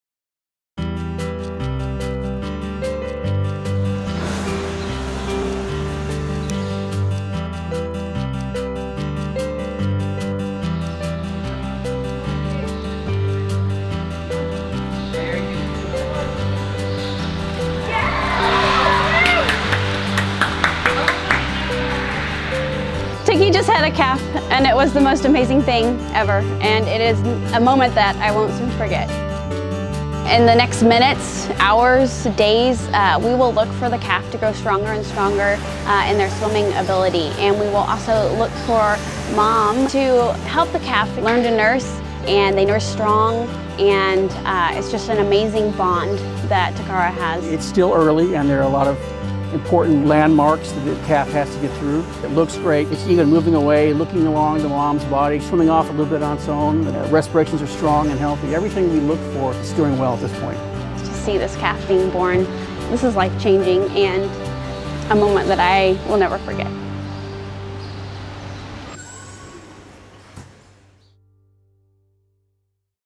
Takara Calf Birth at SeaWorld San Antonio-Nace última orca
Nace la cría de Takara, la última orca en nacer en SeaWorld, en San Antonio. The baby whale is the last one born at SeaWorld.